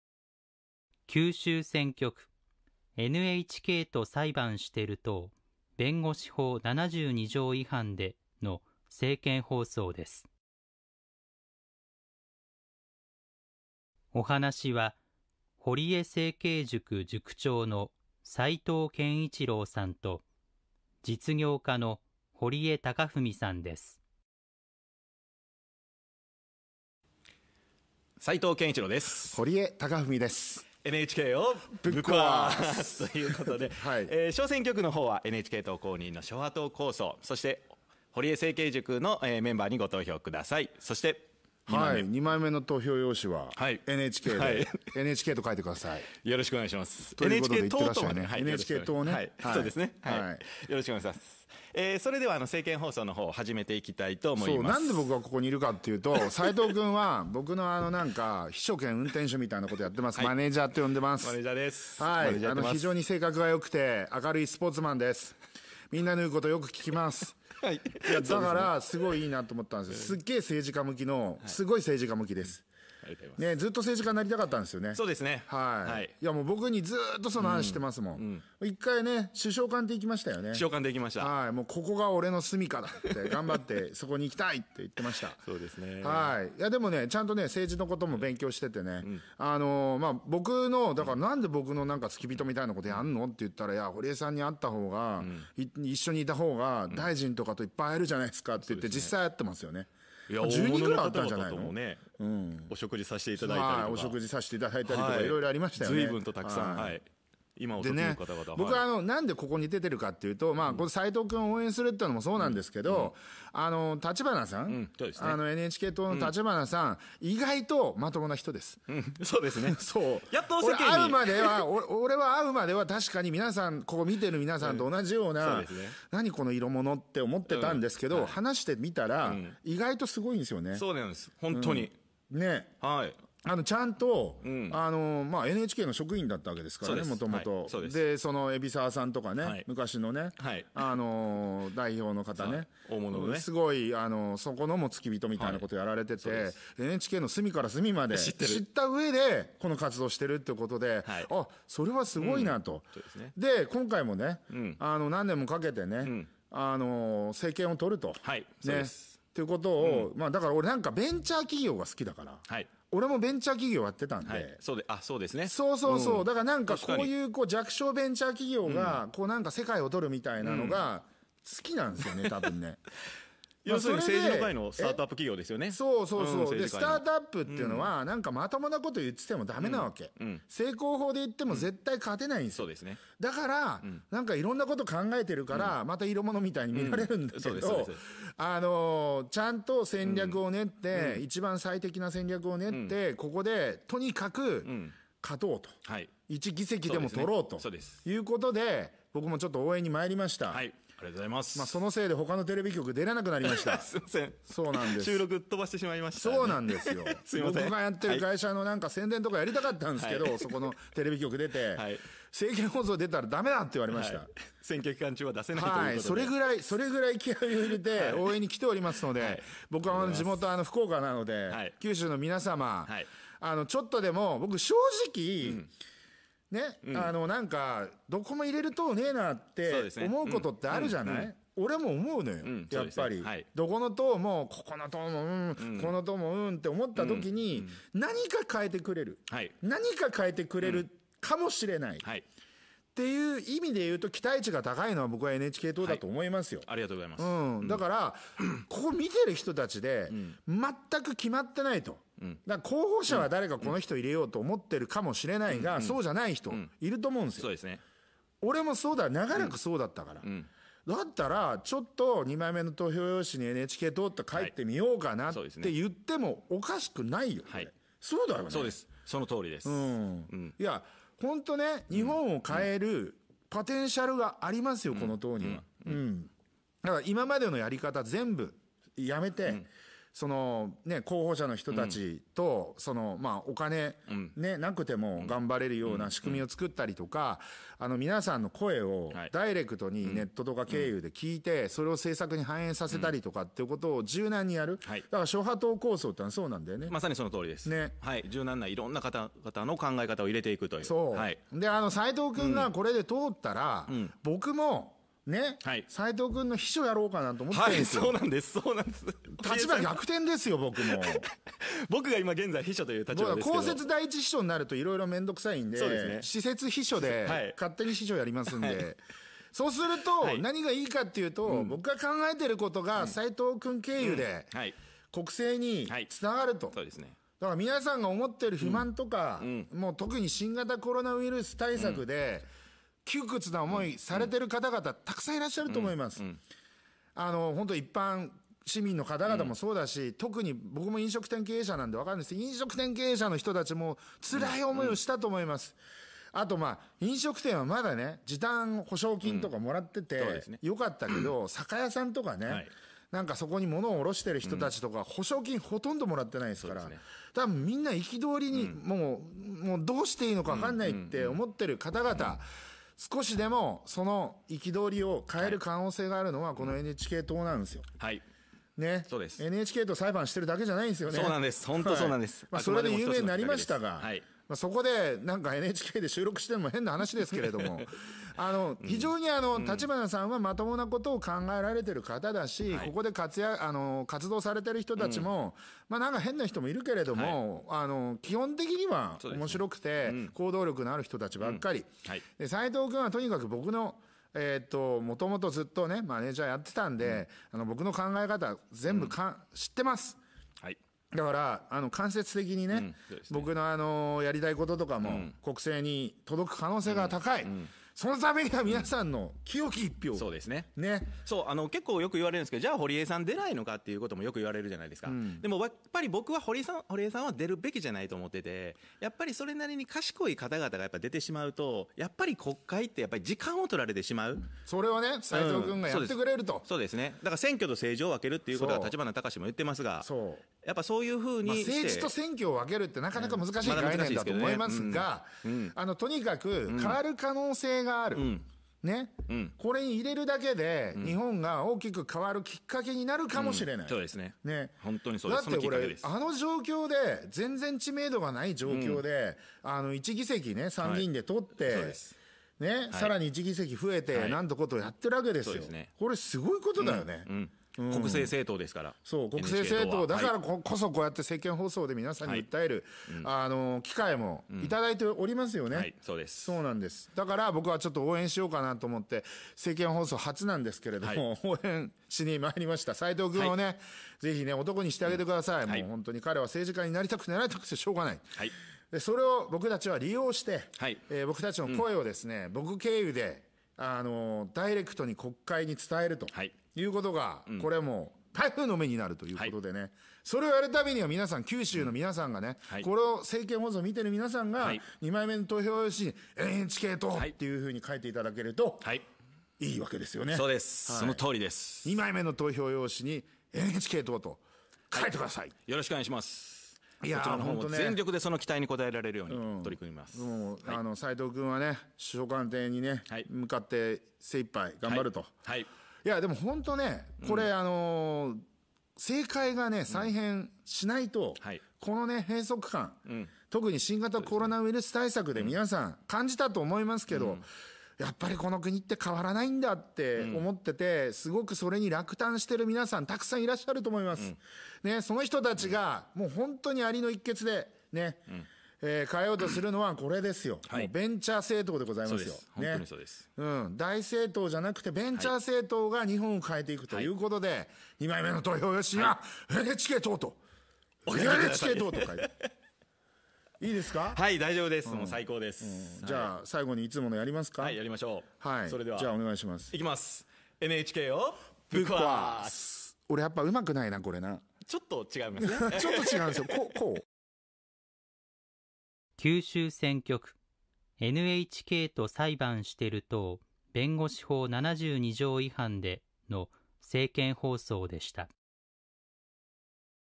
衆院選 九州 比例 さいとう健一郎 堀江貴文 政見放送 NHKと裁判してる党弁護士法72条違反で（NHK党
２３歳のお嫁さん。４０代の私・・・
「件名：速稼塾（あなたのお名前）」